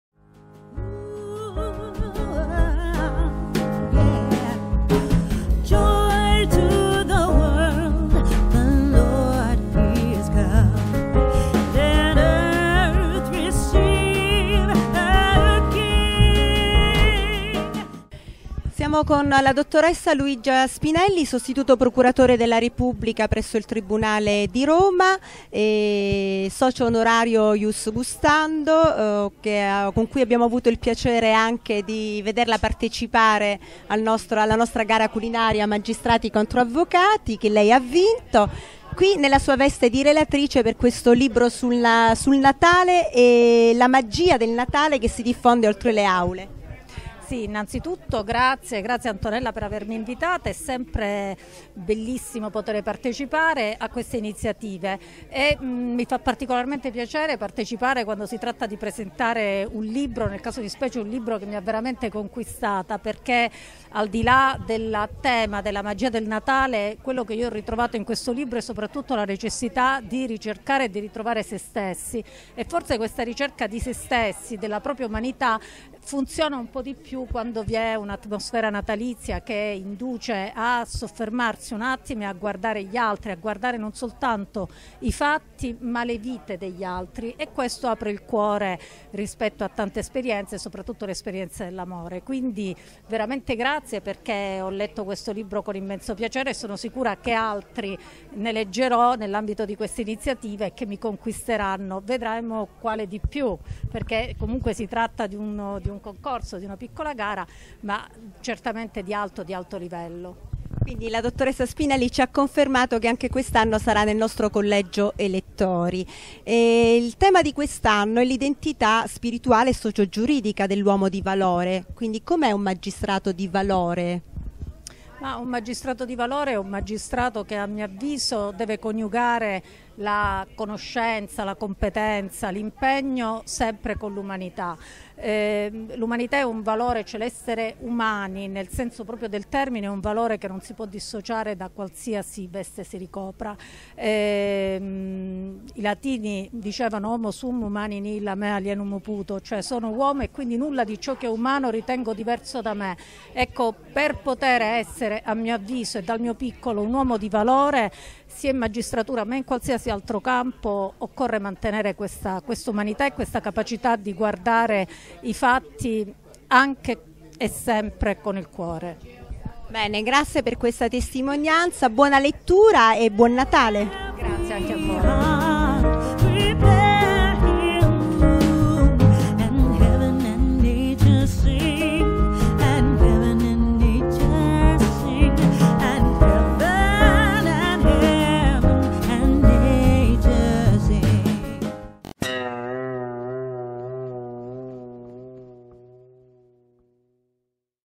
Luigia Spinelli - Presentazione di "Blue Christmas" - Premio IUSARTELIBRI
Presentazione del libro "Blue Christmas" di Raffaella Bonsignori, ediz. Bibliotheka

Intervista di ANTONELLA SOTIRA a LUIGIA SPINELLI, magistrato, Sostituto Procuratore della Repubblica presso il Tribunale di Roma.